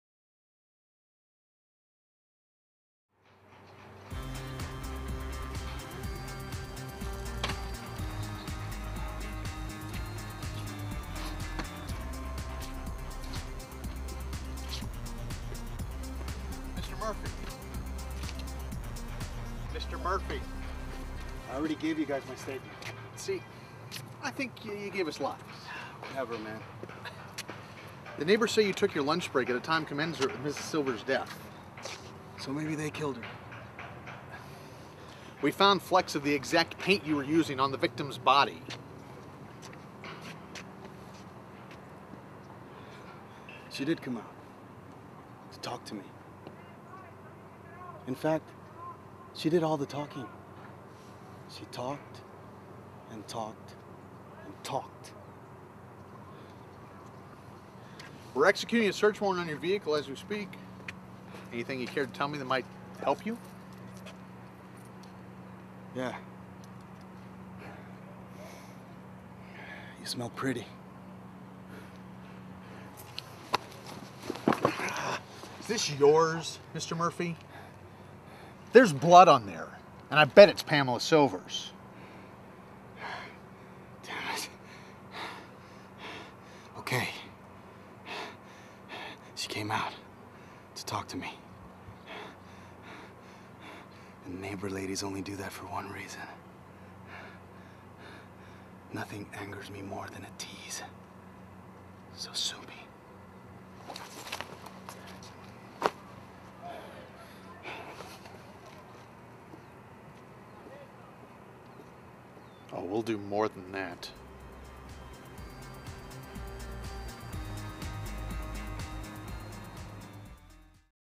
Demo Scene: Cop Drama Paul Rose
Demo Scene - Dramatic Police Procedural type scene with Paul Rose